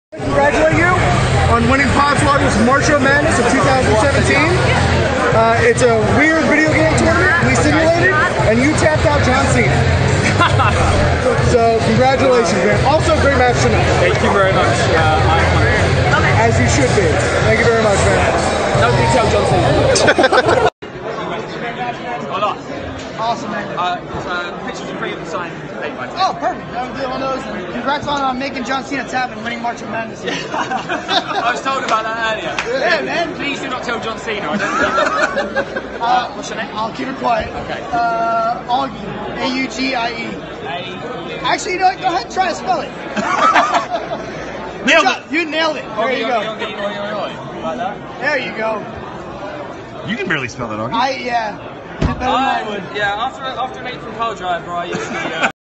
Congratulating Zack Sabre Jr. on WINNING Marcho Madness 2017
It was an honor to personally and continuously congratulate Zack Sabre Jr. on winning the 2017 Marcho Madness Tournament! Be sure to do the same when you see ZSJ and remember, don't tell John!